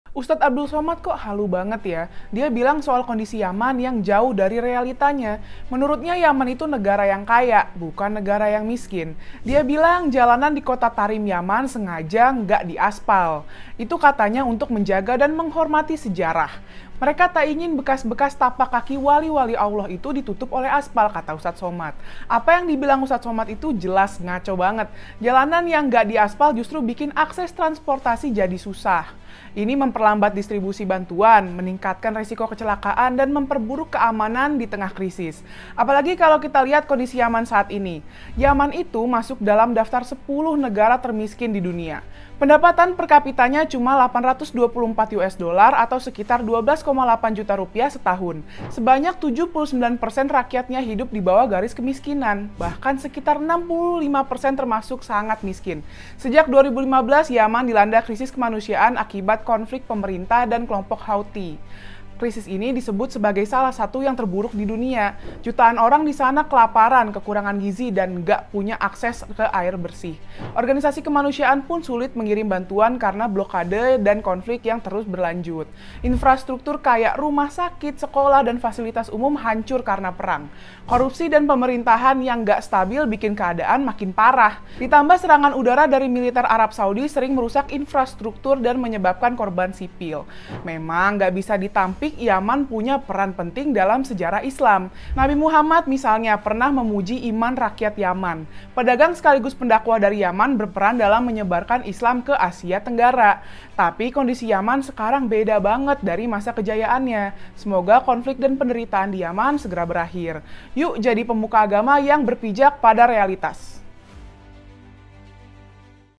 Abdul Somad Bilang Yaman Kaya, Serius?
Abdul Somad Bilang Yaman Kaya, Serius? | Egrasia Salsabila

Baru-baru ini, Ustadz Abdul Somad menyebut Yaman sebagai negara kaya dengan jalanan yang sengaja tidak diaspal untuk menghormati sejarah. Namun, kenyataannya jauh berbeda. Yaman adalah salah satu negara termiskin di dunia, dengan 79% penduduknya hidup di bawah garis kemiskinan. Infrastruktur hancur, dan distribusi bantuan pun semakin sulit. Mari kita jadi pemuka agama yang berpijak pada realitas, bukan ilusi. Semoga konflik dan penderitaan di Yaman segera berakhir🙏

PIS juga punya Website dan Ensiklopedia Islam loh! Yuk, cek disini 👇🏻